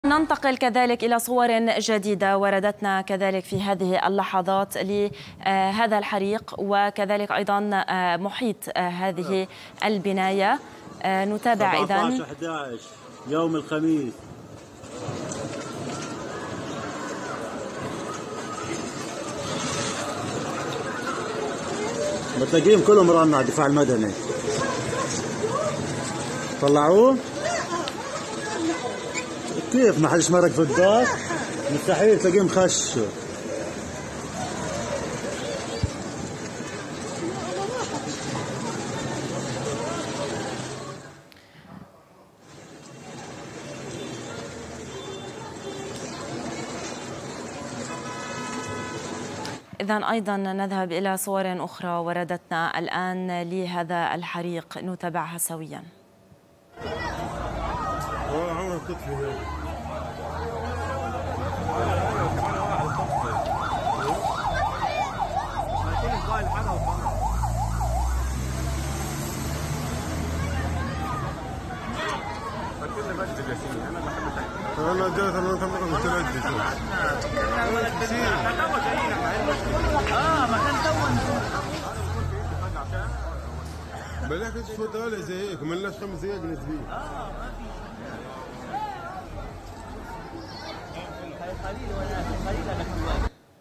صور جديدة لحريق بناية في مخيم جباليا بقطاع غزة
مشاهد جديدة للحريق الذي اندلع في بناية سكنية في مخيم جباليا بقطاع